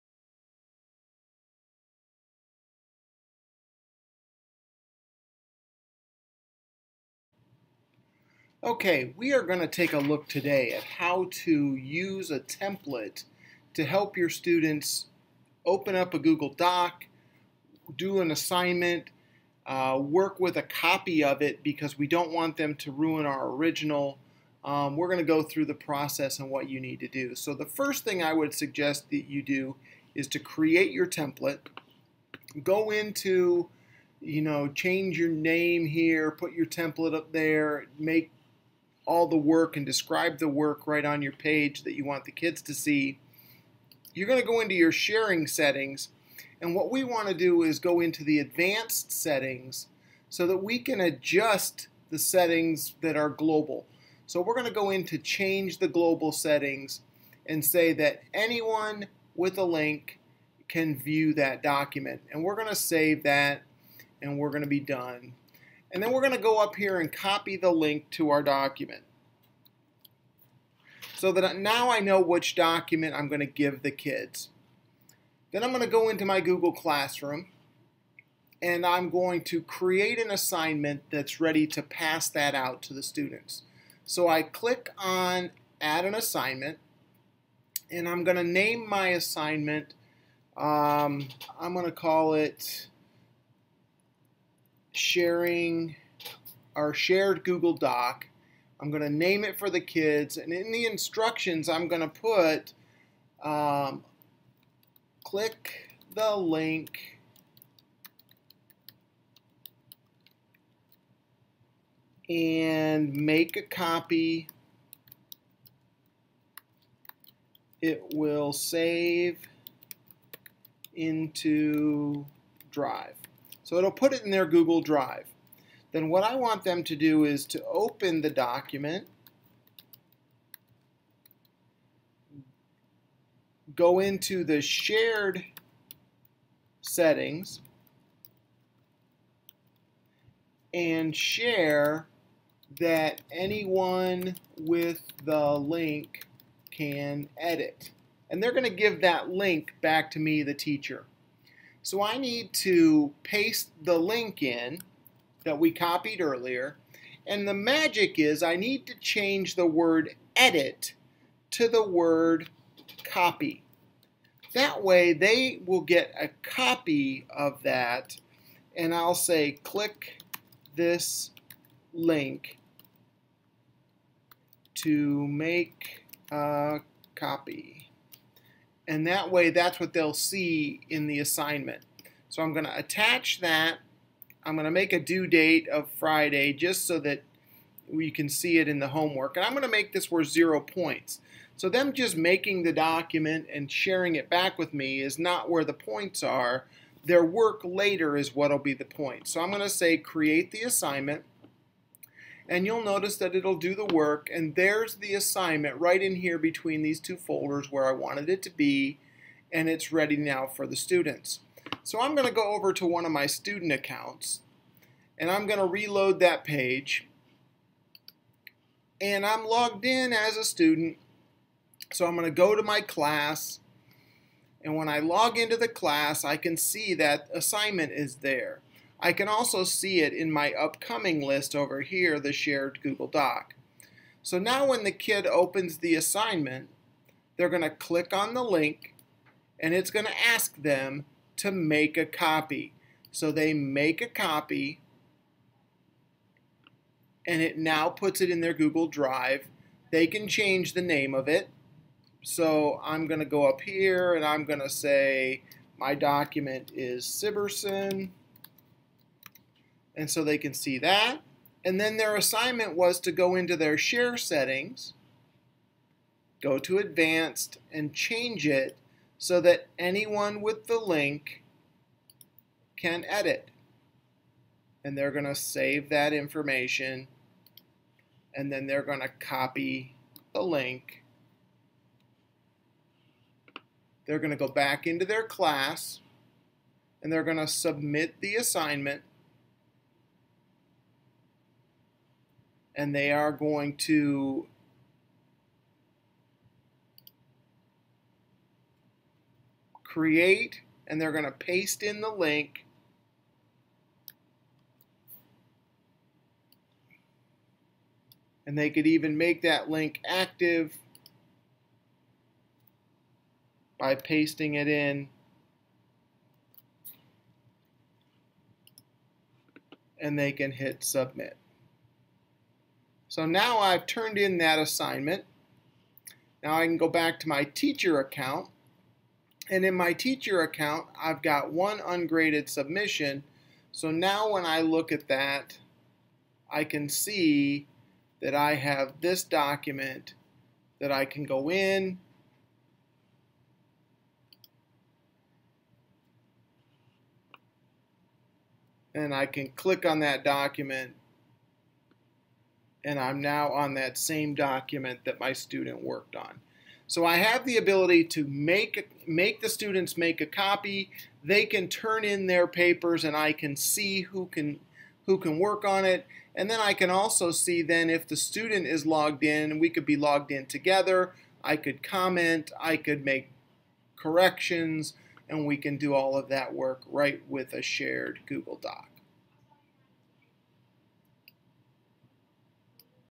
Share a Copy of a Google Doc in Schoology
This video will demonstrate how to use the sharing permissions in Google to share documents and templates with students using Schoology.